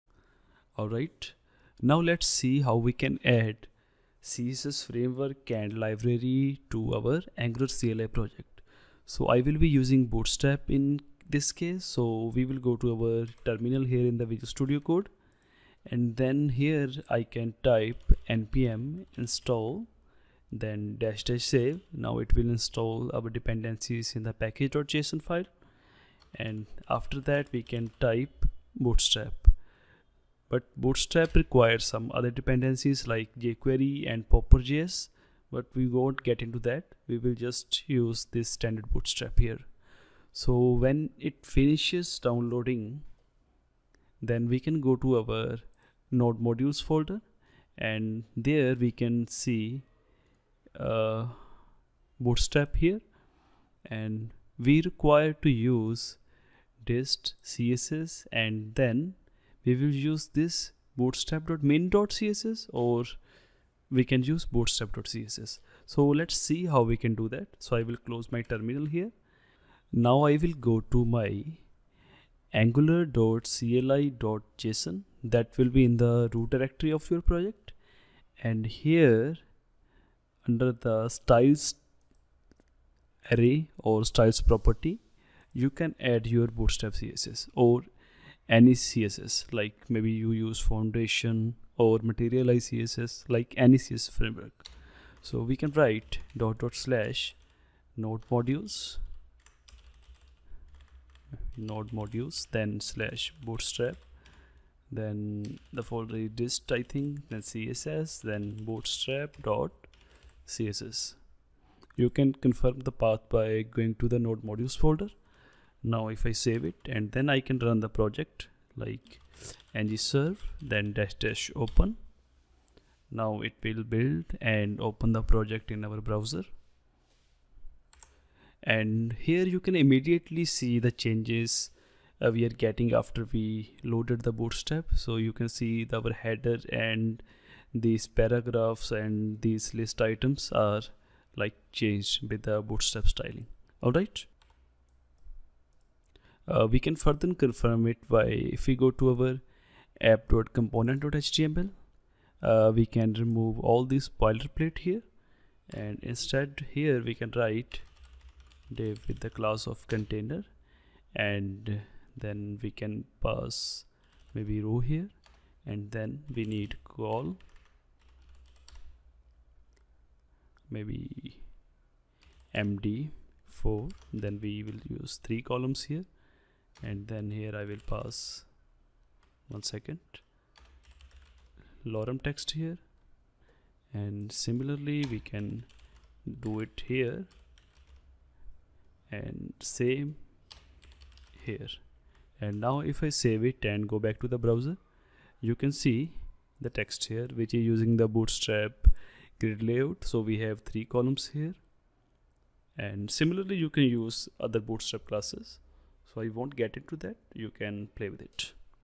Angular CLI [3] - Adding CSS Framework or Library in Project
In this video, we'll see how we can use a CSS framework or library with Angular CLI project. We'll use Bootstrap here.